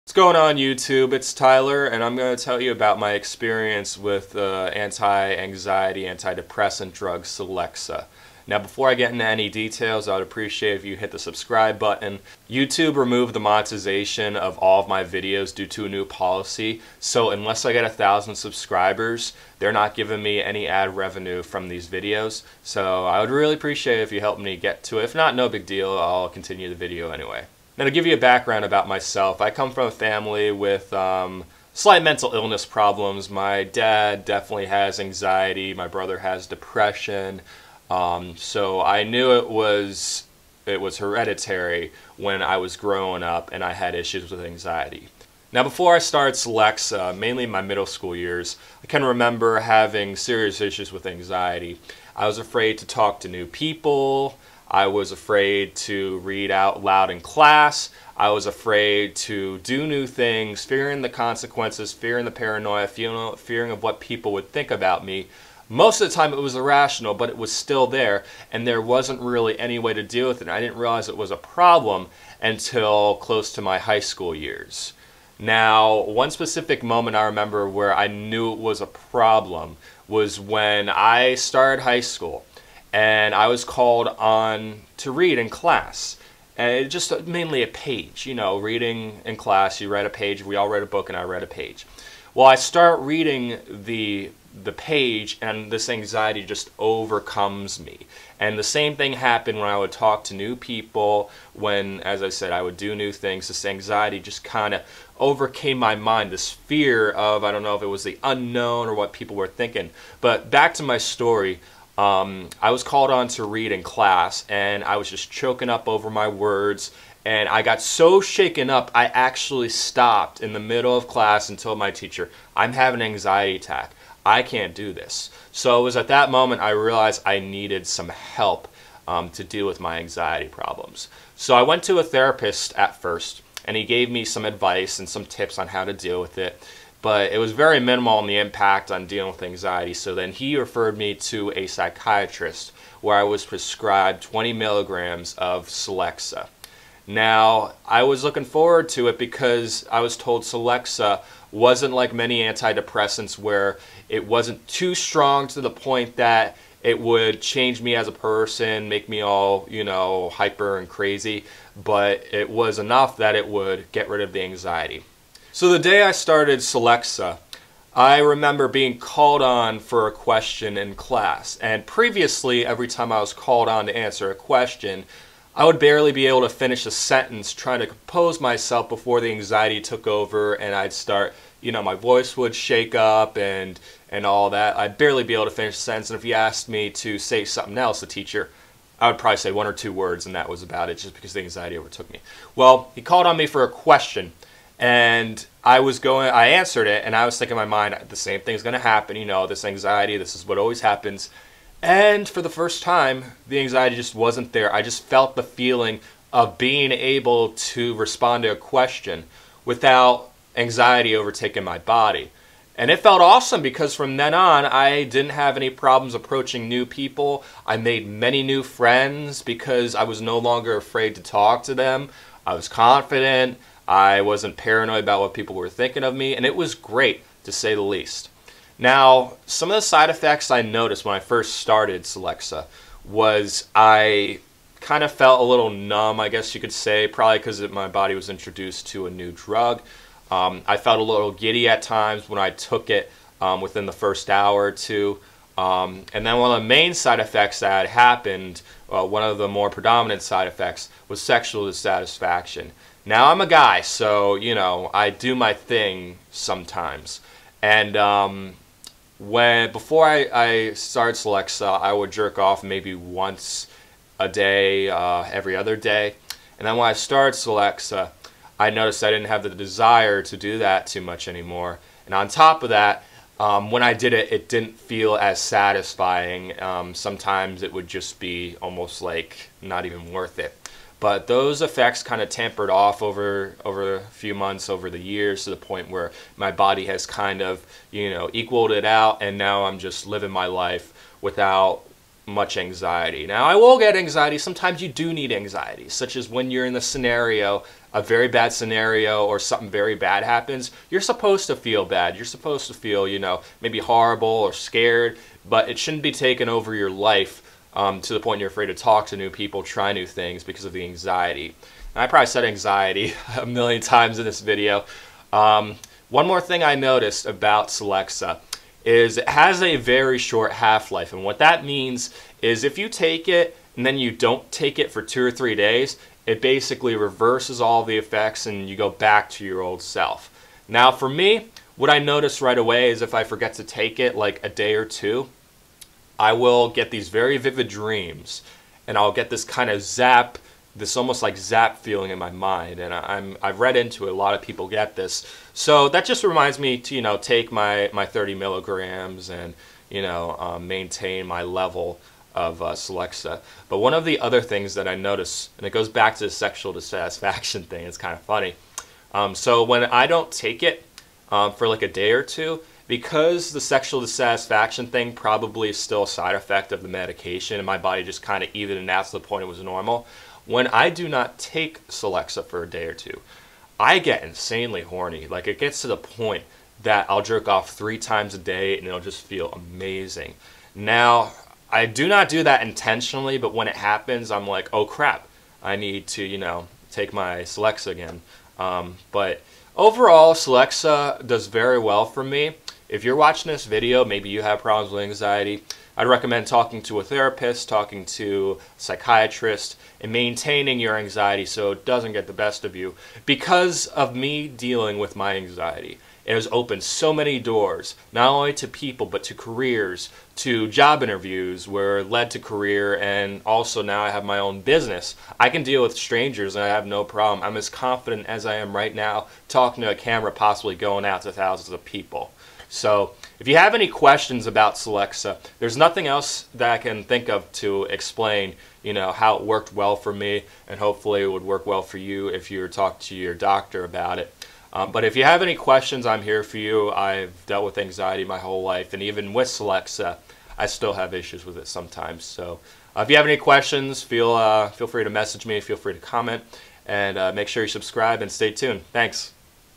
My Experience with Celexa (Citalopram Hydrobomide) and Side effects
My experience with taking the SSRI drug Celexa (citalopram hydrobromide). One side effect I forgot to mention is insomnia if I take it too late in the day.